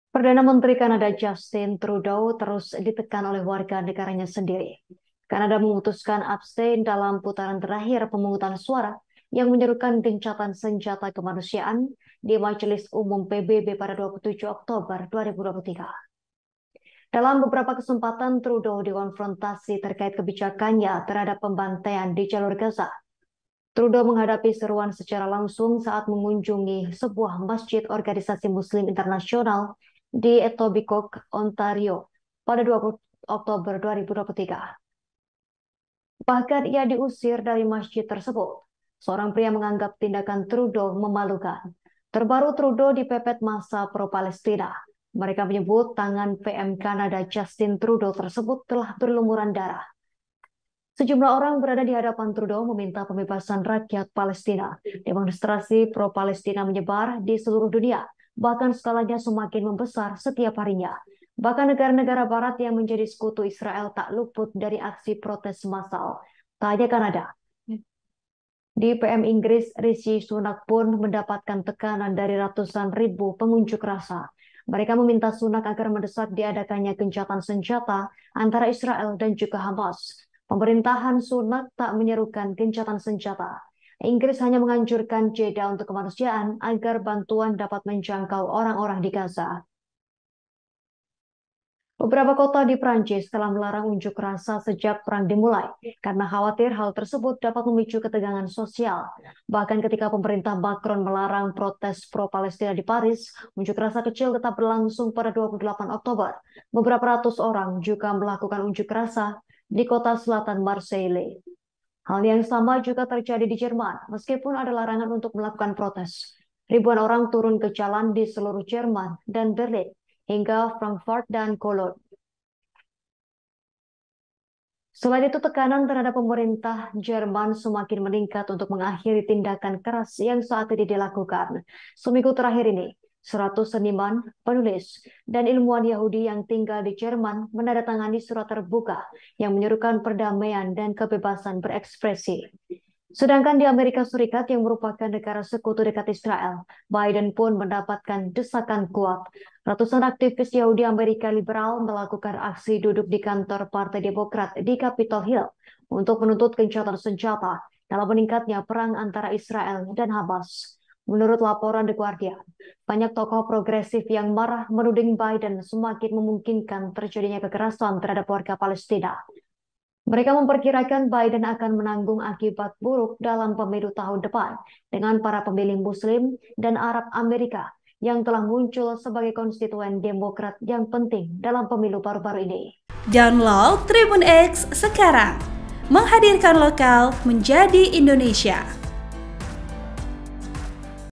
DETIK-DETIK PM Kanada Diusir dari Masjid, Dicap Berlumuran Darah Warga Gaza & Dianggap 'Memalukan'
TRIBUN-VIDEO.COM - Perdana Menteri Kanada, Justin Trudeau terus ditekan oleh warga negaranya sendiri.

Kanada memutuskan abstain dalam putaran terakhir pemungutan suara yang menyerukan 'gencatan senjata' kemanusiaan di Majelis Umum PBB pada 27 Oktober 2023.

Dalam beberapa kesempatan, Trudeau dikonfrontasi terkait kebijakannya terhadap pembantaian di Jalur Gaza.

Trudeau menghadapi seruan secara langsung saat mengunjungi sebuah Masjid Organisasi Muslim Internasional (IMO) di Etobicoke, Ontario, pada 20 Oktober 2023.

Bahkan, ia diusir dari masjid tersebut.

Seorang pria menganggap tindakan Tredau 'memalukan'.

Terbaru, Trudeau dipepet massa pro Palestina.
Mereka menyebut tangan PM Kanada, Justin Tredeau tersebut telah berlumuran darah.

Sejumlah orang yang berada di hadapan Tredau meminta pembebasan rakyat Palestina.

Demonstrasi pro Palestina menyebar di seluruh dunia, bahkan skalanya semakin membesar setiap harinya.

Bahkan negara-negara Barat yang menjadi sekutu Israel tak luput dari aksi protes massal.

Tak hanya Kanada, di PM Inggris, Rishi Sunak pun mendapatkan tekanan dari ratusan ribu pengunjuk rasa.

Mereka meminta Sunak agar mendesak diadakannya gencatan senjata antara Israel dan Hamas.

Pemerintahan Sunak tidak menyerukan gencatan senjata.

Inggris hanya menganjurkan jeda untuk kemanusiaan, agar bantuan dapat menjangkau orang-orang di Gaza.

“Negara-negara adidaya yang terlibat saat ini tidak berbuat cukup.

Inilah sebabnya kami ada di sini, kami menyerukan gencatan senjata, menyerukan hak-hak Palestina, hak untuk hidup, hak untuk hidup, hak asasi manusia, semua hak kami,” kata pengunjuk rasa Camille Revuelta.

Beberapa kota di Perancis telah melarang unjuk rasa sejak perang dimulai, karena khawatir hal tersebut dapat memicu ketegangan sosial.

Bahkan ketika pemerintah Macron melarang protes pro-Palestina di Paris, unjuk rasa kecil tetap berlangsung pada 28 Oktober. Beberapa ratus orang juga melakukan unjuk rasa di kota selatan Marseille.

Hal yang sama juga terjadi di Jerman. Meskipun ada larangan untuk melakukan protes, ribuan orang turun ke jalan di seluruh Jerman, dari Berlin hingga Frankfurt dan Cologne.

Selain itu, tekanan terhadap pemerintah Jerman semakin meningkat untuk mengakhiri tindakan keras yang saat ini dilakukan.

Seminggu terakhir ini, 100 seniman, penulis, dan ilmuwan Yahudi yang tinggal di Jerman menandatangani surat terbuka yang menyerukan perdamaian dan kebebasan berekspresi.

Sedangkan di AS yang merupakan negara sekutu dekat Israel, Biden pun mendapatkan desakan kuat.
Ratusan aktivis Yahudi Amerika liberal melakukan aksi duduk di kantor Partai Demokrat di Capitol Hill untuk menuntut gencatan senjata dalam meningkatnya perang antara Israel dan Hamas.

Menurut laporan The Guardian, banyak tokoh progresif yang marah, menuduh Biden semakin memungkinkan terjadinya kekerasan terhadap warga Palestina.

Mereka memperkirakan Biden akan menanggung akibat buruk dalam pemilu tahun depan dengan para pemilih Muslim dan Arab-Amerika yang telah muncul sebagai konstituen Demokrat yang penting dalam pemilu baru-baru ini.

(Tribun-Video.com/ Timesofisrael.com)